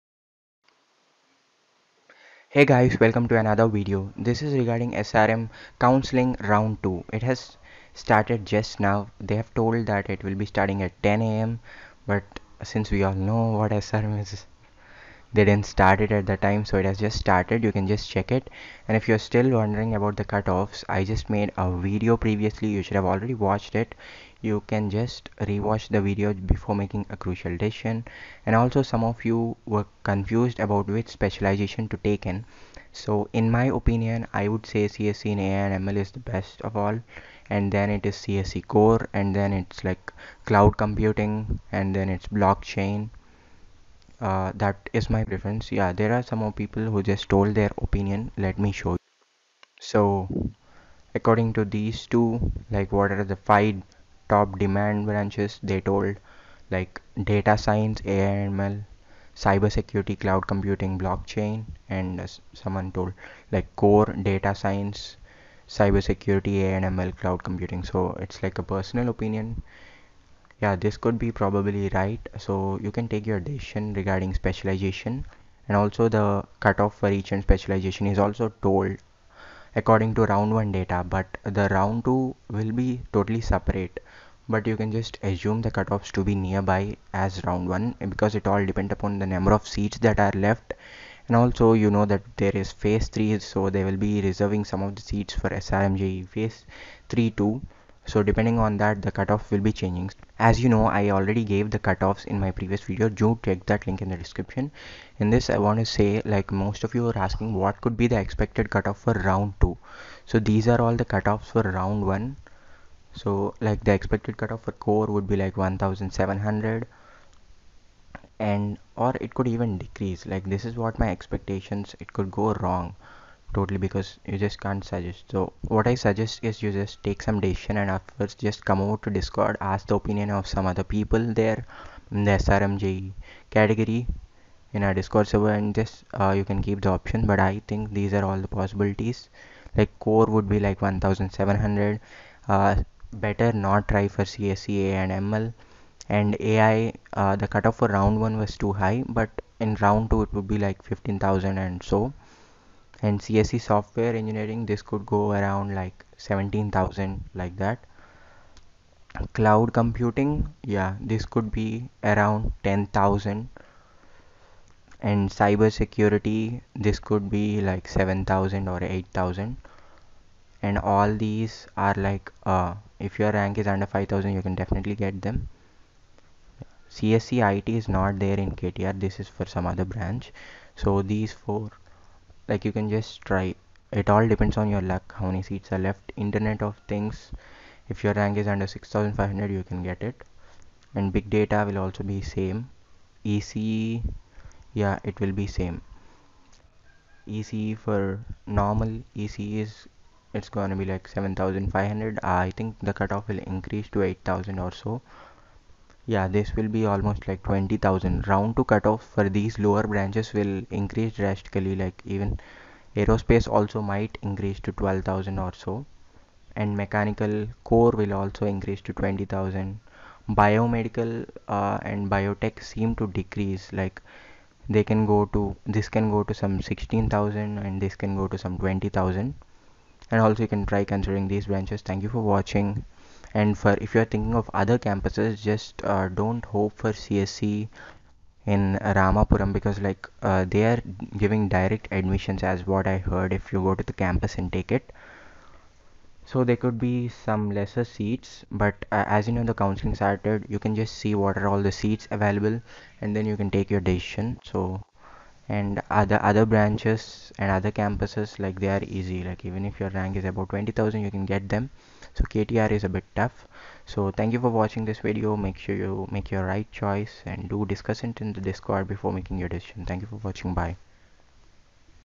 SRM Phase 2 Round 2 counselling | Expected Cut off for round 2
Expected cutoff for round 2 ( personal openion , as most of you were forsing me to say) but better check Original round 1 cut off details of my video )
Round 1 Cut off video ( Must watch )

0:00 Srm councelling round 2
0:27 which spcilisation in cse ktr
2:00 Expected cutoff round 2

SRMJEEE Phase 2 Closing ranks Cut off Rank vs Branch and specialisations